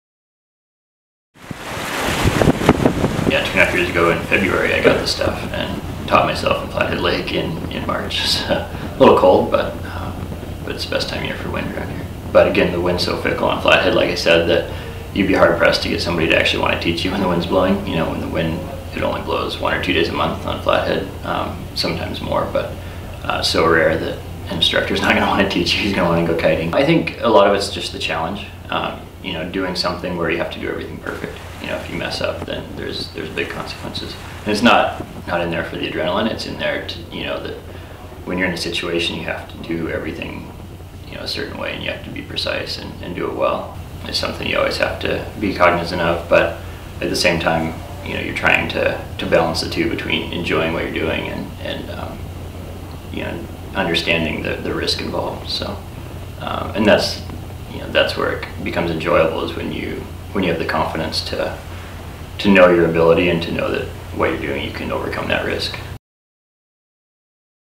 Kiteboarding on Flathead Lake
Brandon French embraces windy weather to kiteboard on Flathead Lake